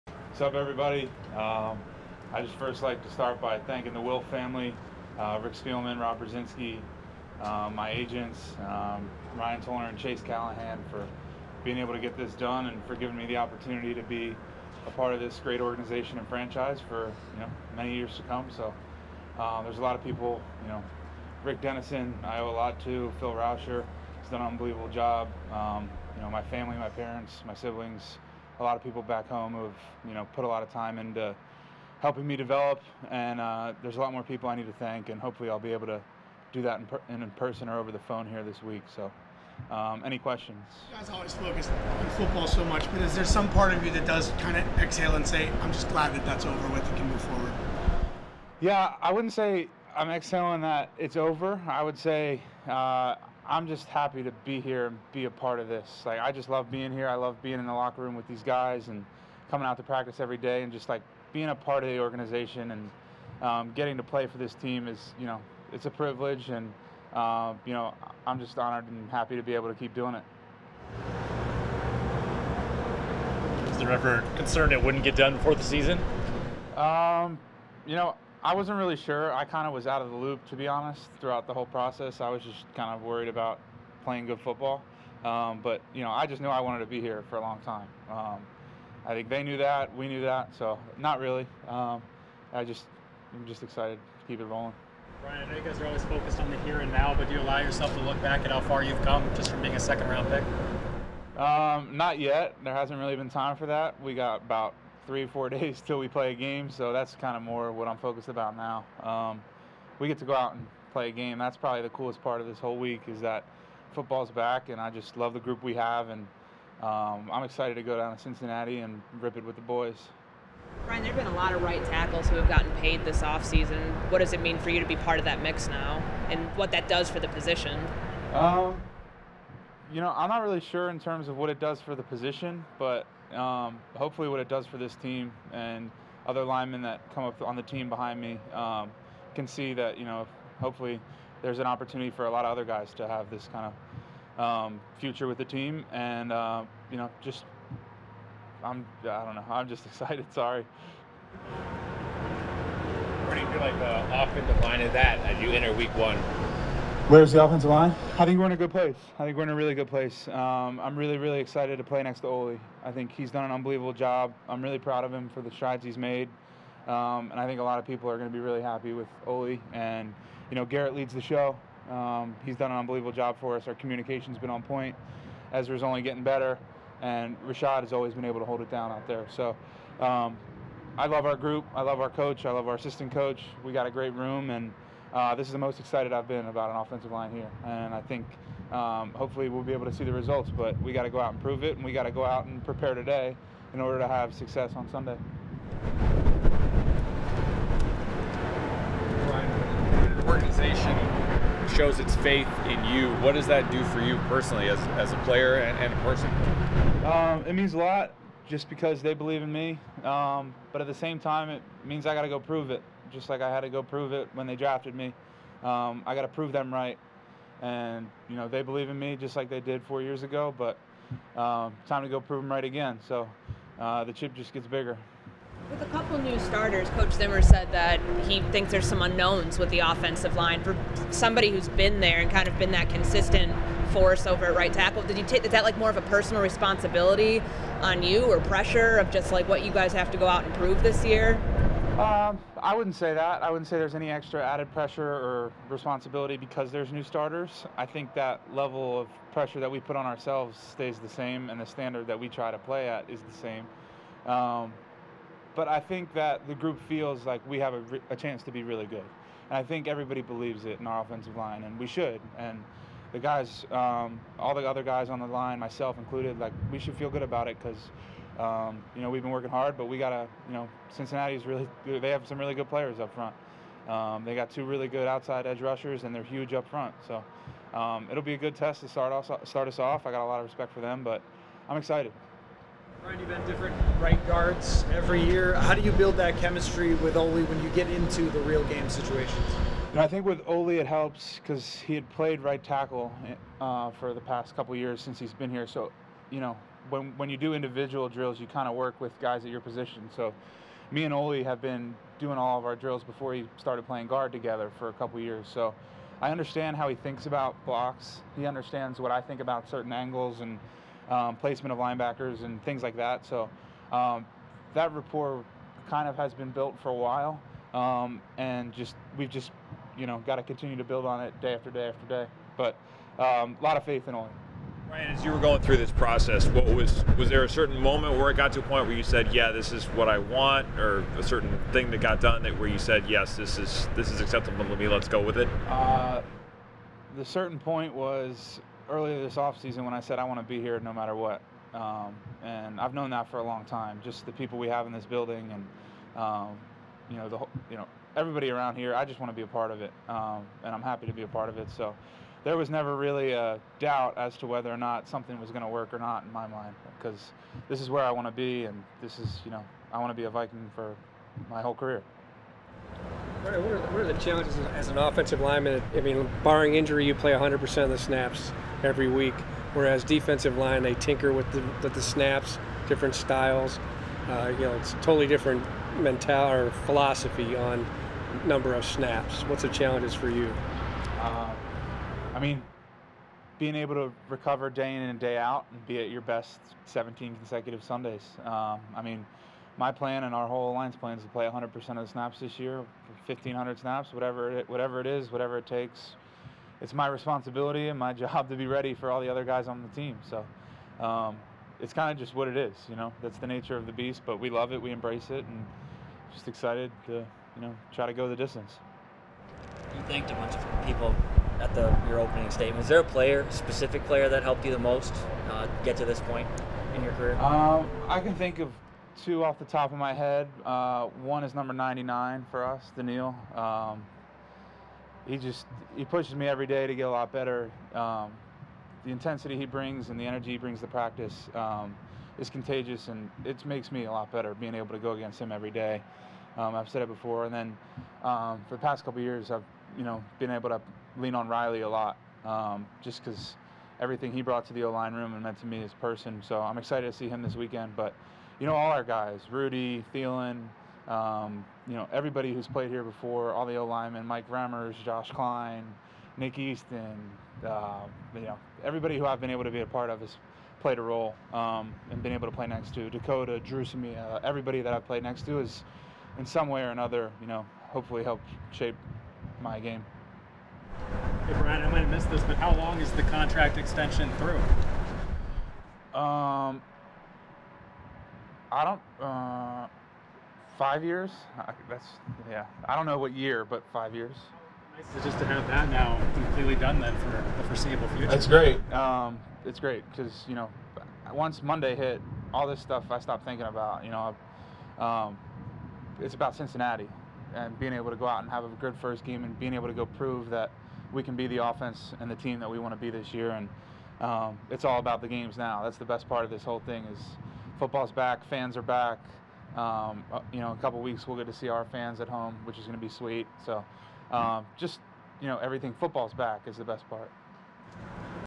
Brian O'Neill on His Contract Extension: Getting To Play For This Team Is A Privilege
T Brian O'Neill addressed the media from the TCO Performance Center on Wednesday.